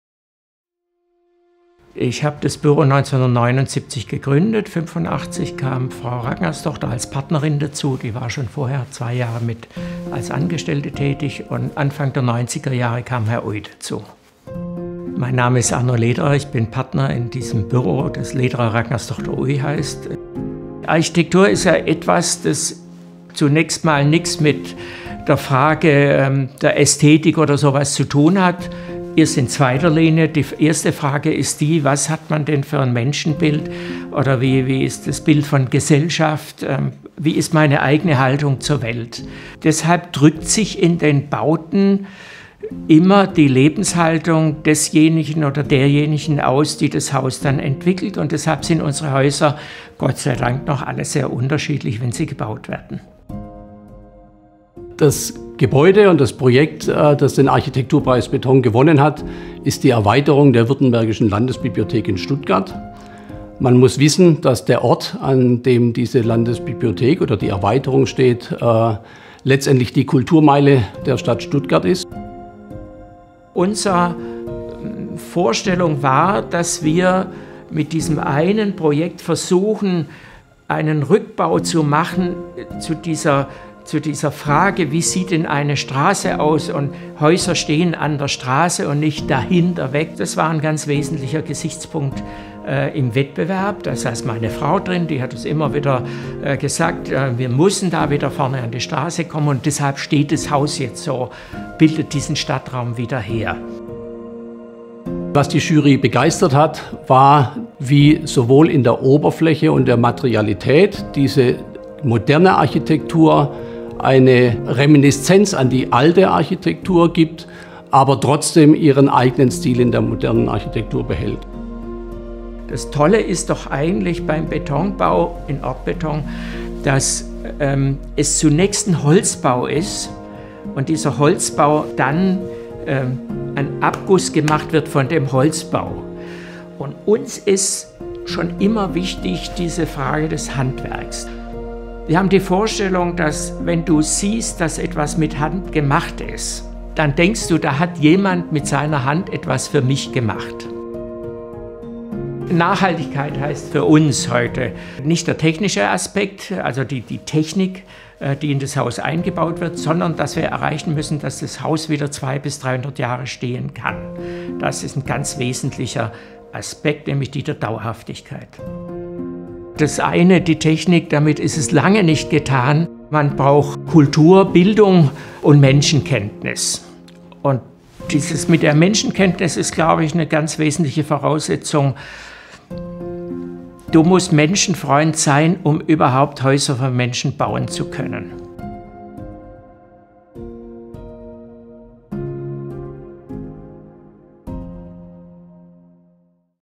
Interview mit Arno Lederer
Präsentation der Gewinner des Architekturpreis Beton 2020:
Erweiterung der Württembergischen Landesbibliothek Stuttgart, Architekturbüro Lederer Ragnarsdóttir Oei